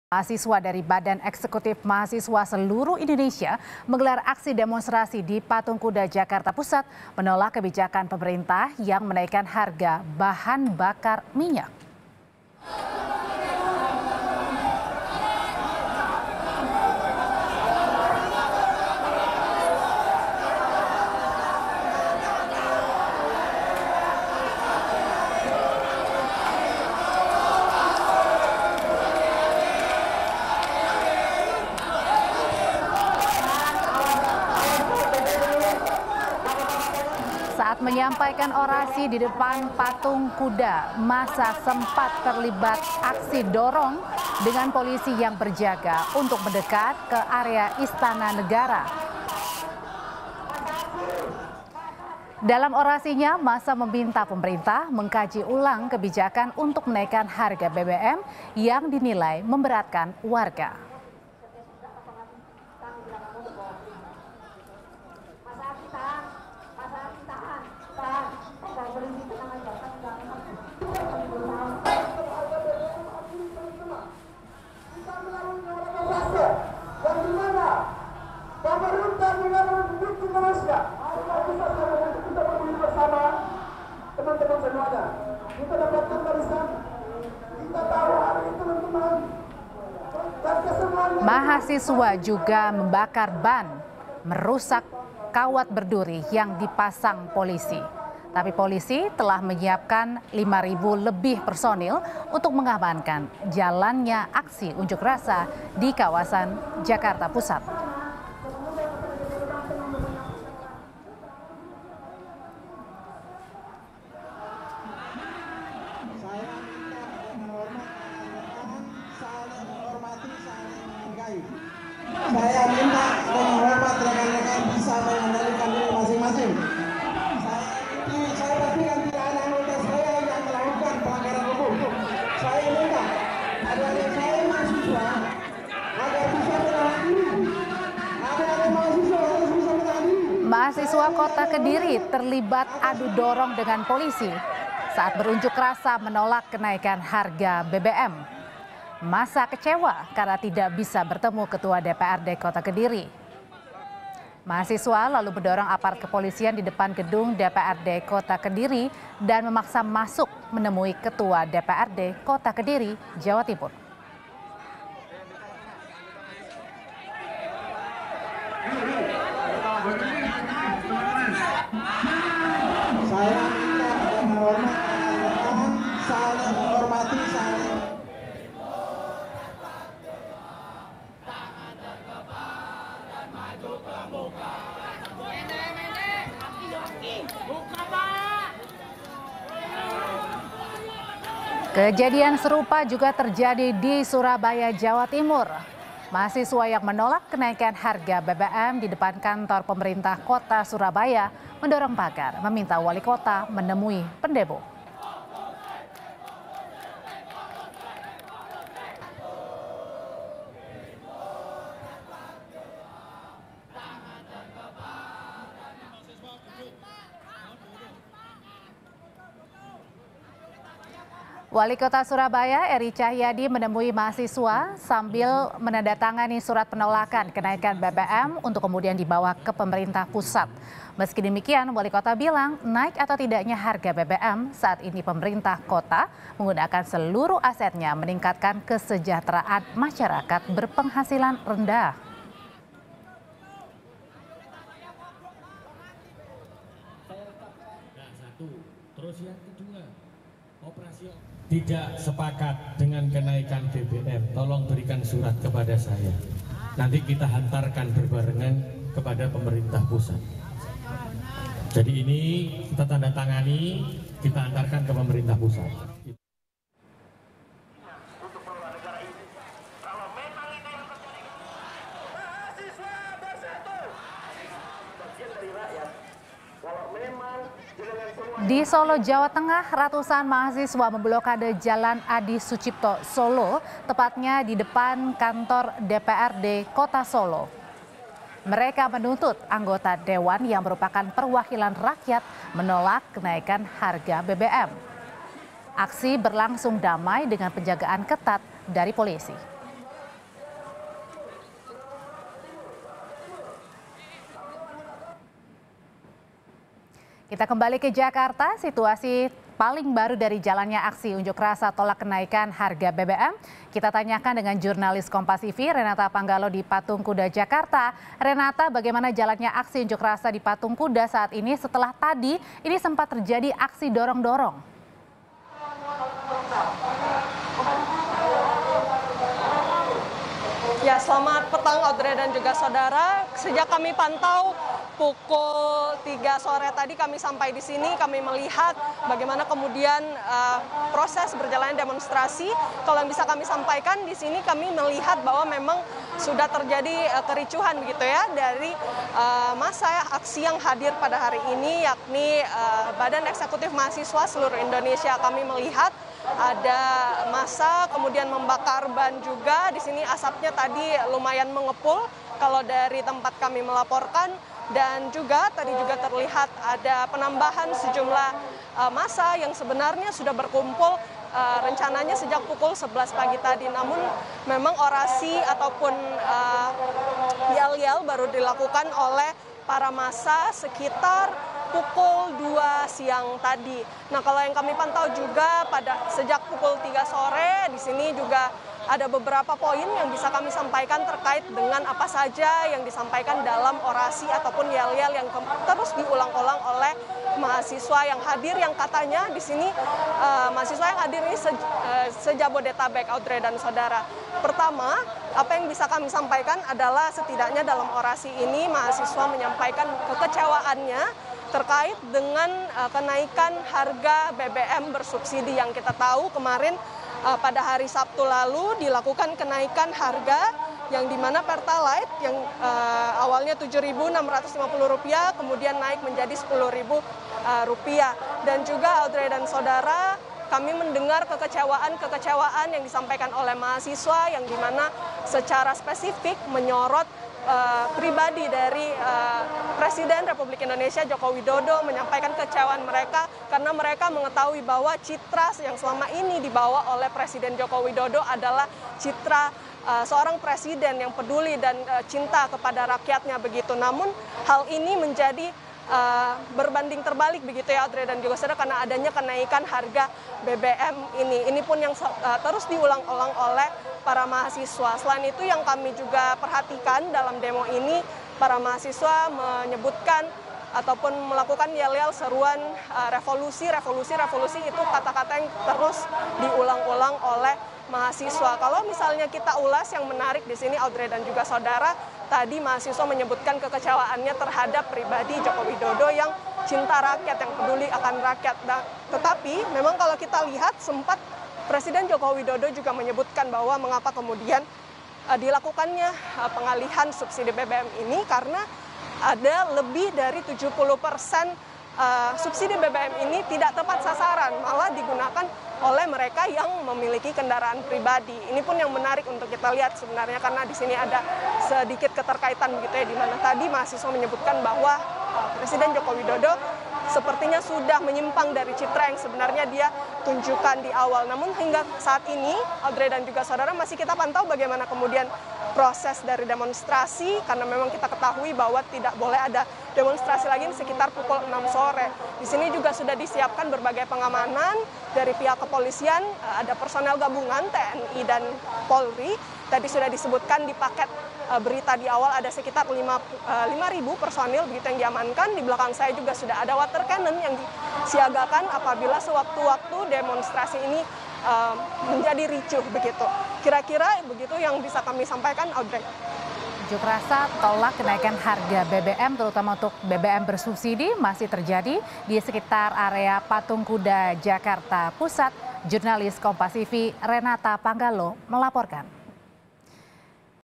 Aksi BEM SI Jebol Pagar Kawat Hingga Wali Kota Surabaya Tanda Tangani Surat Tolak Kenaikan BBM
JAKARTA, KOMPAS.TV - Massa dari Badan Eksekutif Mahasiswa Seluruh Indonesia menggelar aksi demonstrasi di Patung Kuda, Jakarta Pusat menolak kebijakan pemerintah yang menaikan harga bahan bakar minyak.

Saat menyampaikan orasi di depan Patung Kuda, massa sempat terlibat aksi dorong dengan polisi yang berjaga untuk mendekat ke area Istana Negara.

Dalam orasinya massa meminta pemerintah untuk mengkaji ulang kebijakan untuk menaikan harga BBM yang dinilai memberatkan warga.

Mahasiswa membakar ban dan merusak kawat berduri yang dipasang polisi.

Namun polisi telah menyiapkan 5800 personel untuk mengamankan jalannya aksi unjuk rasa di kawasan Jakarta Pusat.

Ratusan mahasiswa Kota Kediri terlibat adu dorong dengan polisi saat berunjuk rasa menolak kenaikan harga BBM.

Massa kecewa karena tidak bisa bertemu Ketua DPRD Kota Kediri.

Mahasiswa mendorong aparat kepolisian di depan Gedung DPRD Kota Kediri.

Massa memaksa masuk, menemui Ketua DPRD Kota Kediri, Jawa Timur.

Kejadian serupa juga terjadi di Surabaya, Jawa Timur.

Mahasiswa yang menolak kenaikan harga BBM di depan kantor pemerintah Kota Surabaya, mendorong pagar, meminta wali kota menemui pendemo.

Wali Kota Surabaya, Eri Cahyadi menemui mahasiswa sambil menandatangani surat penolakan kenaikan bbm untuk dibawa ke pemerintah pusat.

Meski demikian, wali kota bilang naik atau tidaknya harga BBM, saat ini pemerintah kota menggunakan seluruh asetnya meningkatkan kesejahteraan masyarakat berpenghasilan rendah.

Ratusan mahasiswa memblokade Jalan Adisucipto Solo, Jawa Tengah, tepat di depan Kantor DPRD Kota Solo.

Mereka menuntut anggota dewan yang merupakan perwakilan rakyat, untuk menolak kenaikan harga BBM.

Aksi berlangsung damai, dengan penjagaan ketat dari kepolisian.